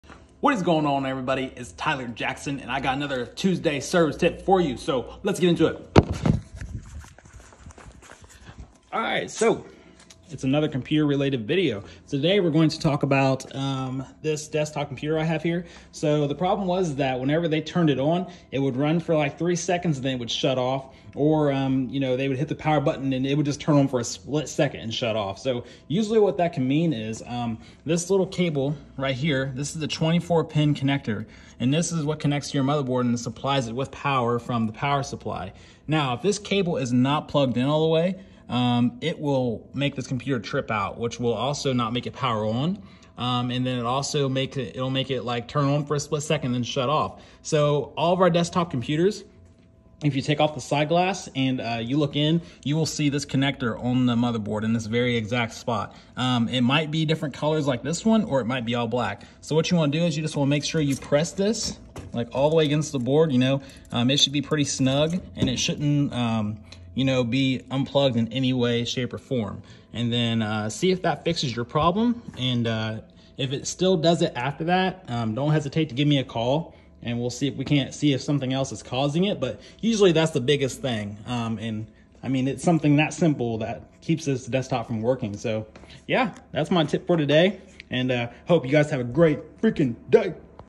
Desktop Computer Troubleshooting- Unit powers on then shuts off
Check out this video if you have a desktop computer that is turning on but shutting off right after.